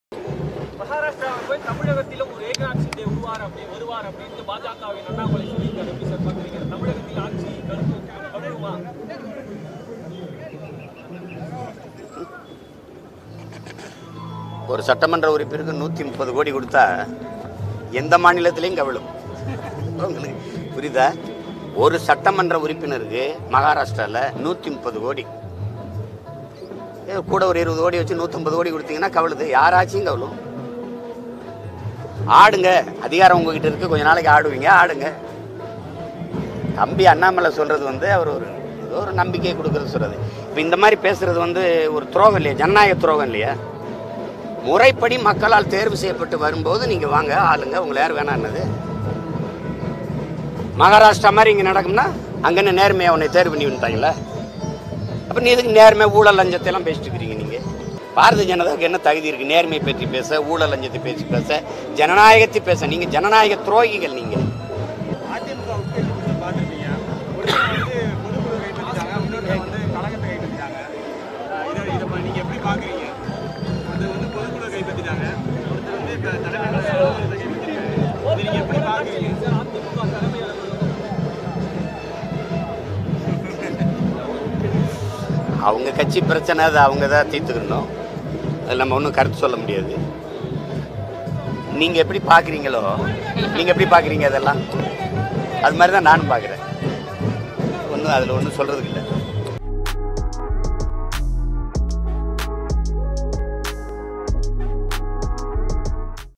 நேர்மை, ஊழல் பற்றி பேச பாஜகவுக்கு தகுதி இல்லை..! சீமான் / Naam Tamilar Katchi / BJP / Seeman
நேர்மை, ஊழல் பற்றி பேச பாஜகவுக்கு தகுதி இல்லை..! சீமான் / Naam Tamilar Katchi / BJP / Seeman / MK Stalin / Annamalai